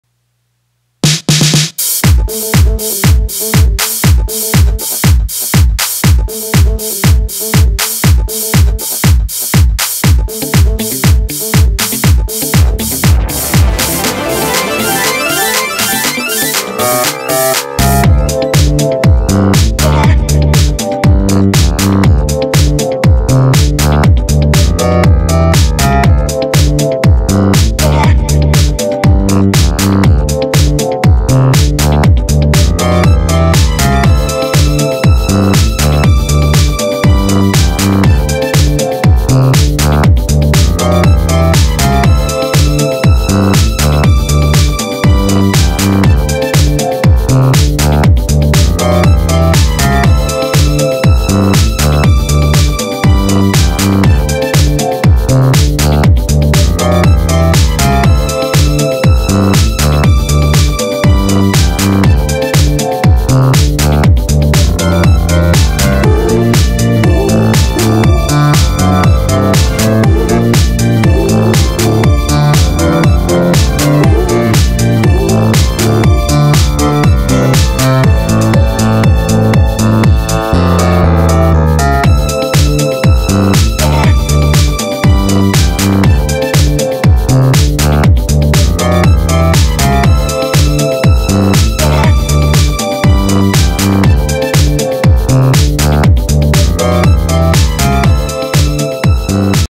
mini mutsen presentation
mini mutsen at Collage Spektakel Volkskrantgebouw & Canvas op de 7de, Amsterdam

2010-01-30